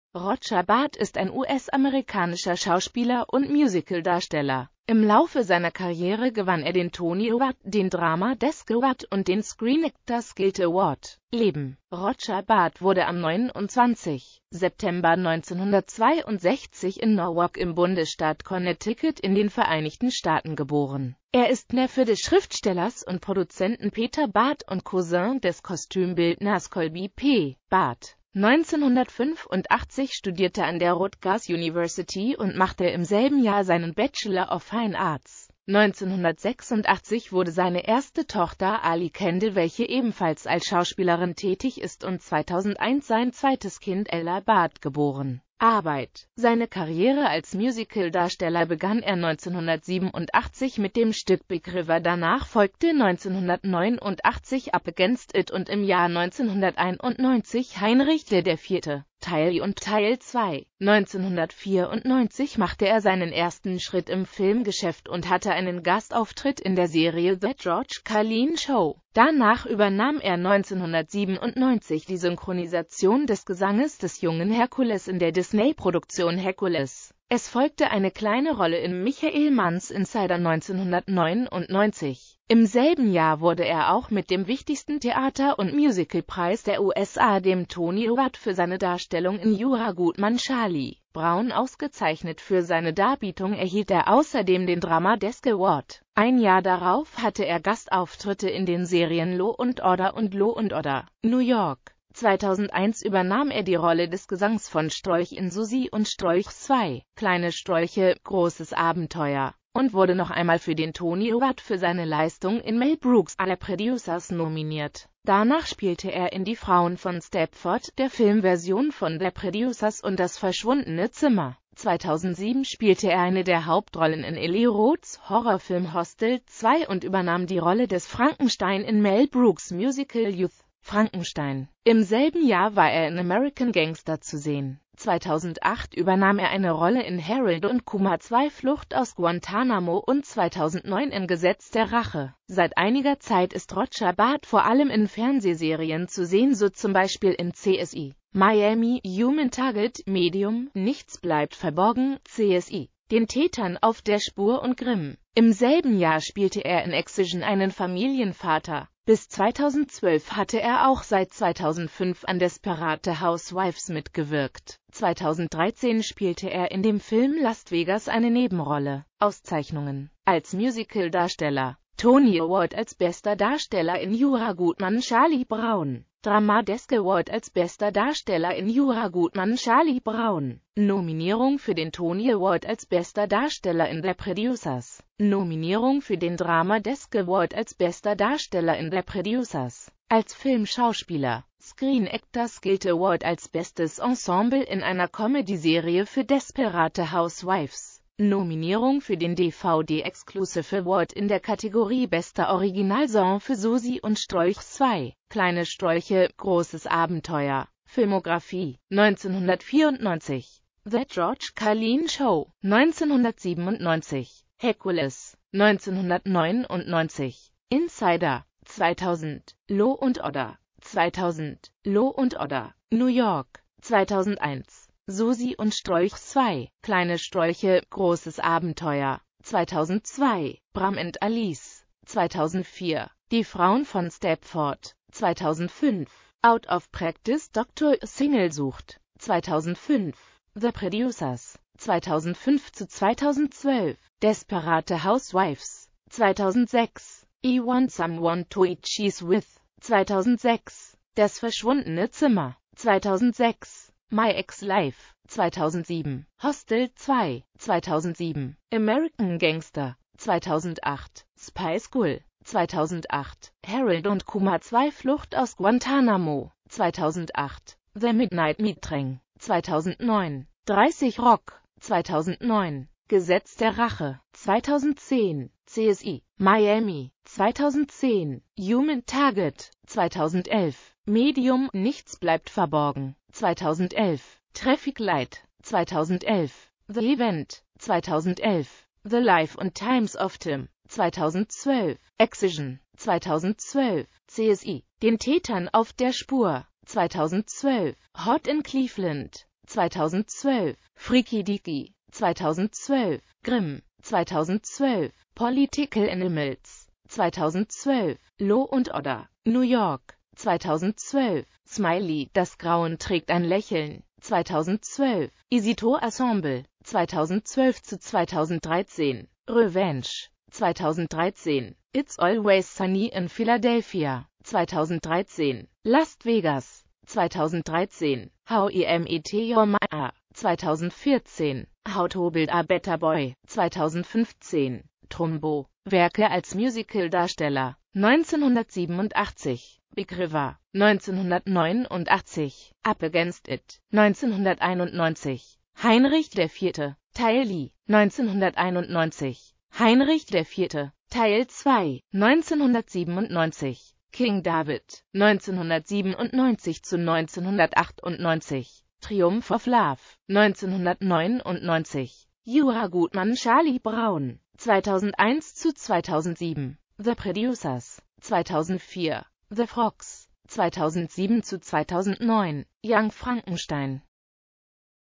Roger Bart
Vielen Dank für Ihre Unterstützung:

Roger Bart (* 29.September 1962 in Norwalk, Connecticut) ist ein US-amerikanischer Schauspieler und Musicaldarsteller.Im Laufe seiner Karriere gewann er den Tony Award, den Drama Desk Award und den Screen Actors Guild Award.